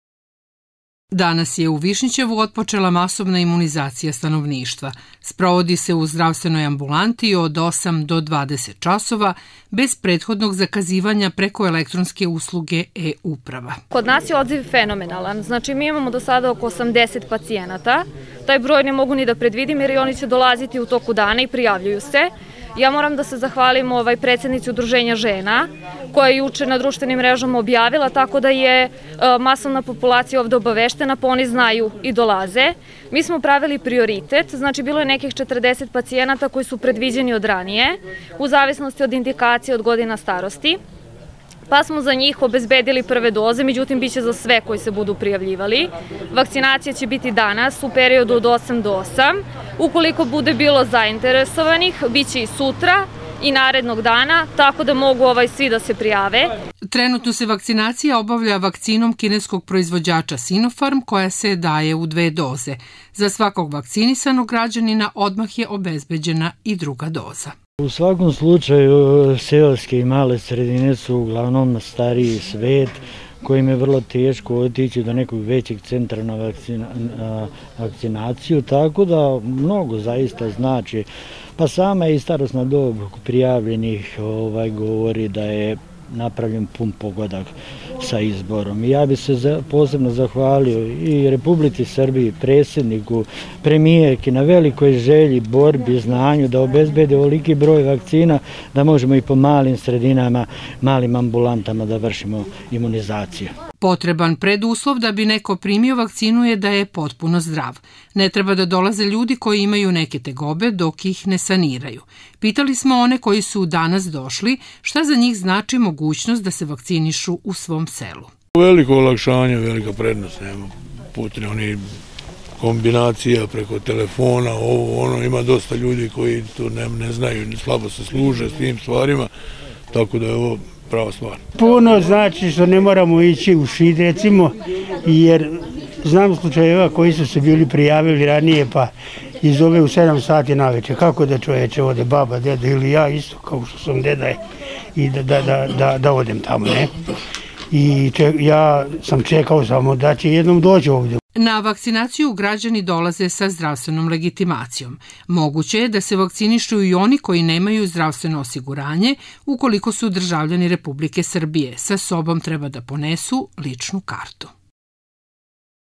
Masovna imunizacija u Višnjićevu
Od danas, u Domu zdravlja Šid, počinje vakcinacija građana bez prethodnog zakazivanja E-upravi. Svi zdravi građani koji žele da se vakcinišu kineskom „Sinofarm” vakcinu potrebno je da dođu u Dom zdravlja i sa sobom ponesu zdravstvenu knjižicu. Građani koji nemaju zdravstveno osiguranje, a državljani su Republike Srbije, treba da imaju ličnu kartu. Od danas meštani Višnjićeva imaće mogućnost da se vakcinišu u svom selu, a od sutra kreće imunizacija u sledećim mesnim zajednicama: Jamena, Morović, Sot, Kukujevci, Erdevik i Vašica. U narednom periodu masovna vakcinacija vršiće će u svim seoskim zdravstvenim ambulantama prema rasporedu Štaba za vanredne situacije opštine Šid.